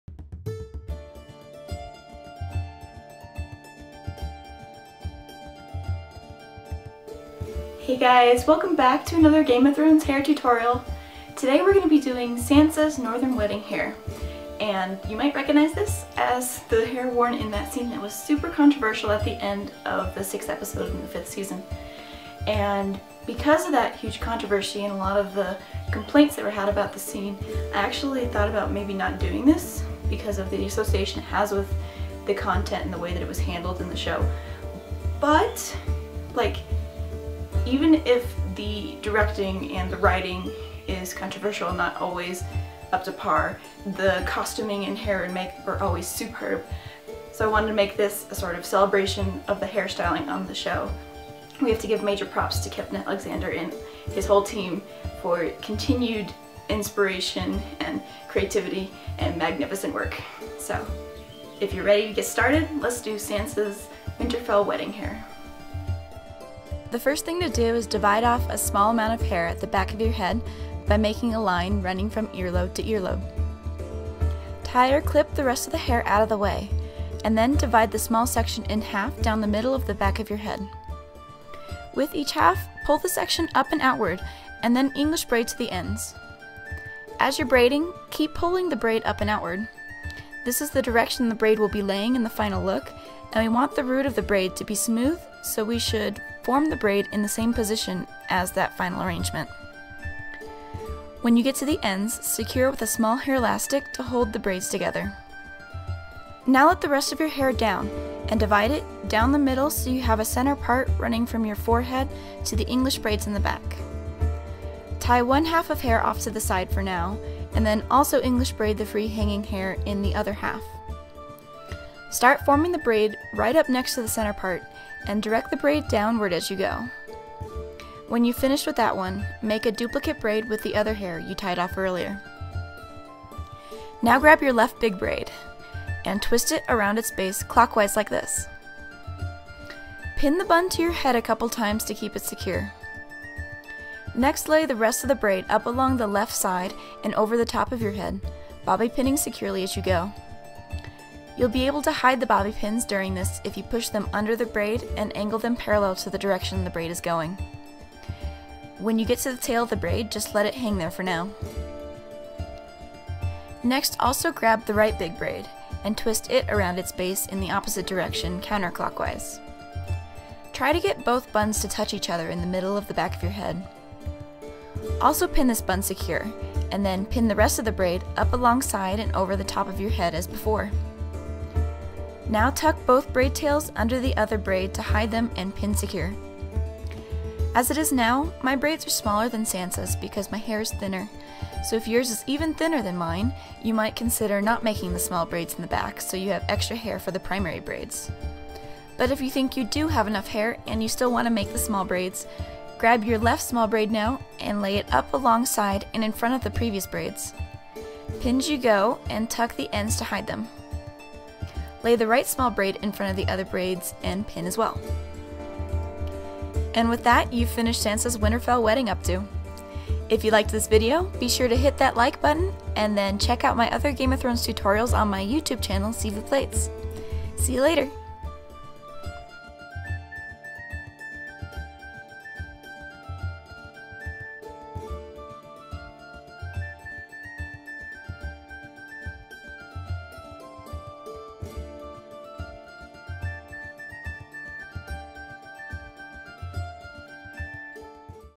Game of Thrones - How To for Sansa's Winterfell Wedding Updo
Interesting that such a beautiful hairdo was featured in such a controversial scene, but that's Game of Thrones for you isn't it? Here is a tutorial for the gorgeous updo worn by Sansa in season 5, during the wedding at Winterfell.

Difficulty: Moderately Easy
Hair Length: at least mid-torso
Materials: bobbypins, 4 small hair elastics
Skills needed: English braid
Time cost: 15-40 minutes
Ideal hairtype: any

I have a new website! Visit silvousplaits.com to see tutorial videos with more text instructions, pictorials, inspiration pictures, blog entries, and other styling services!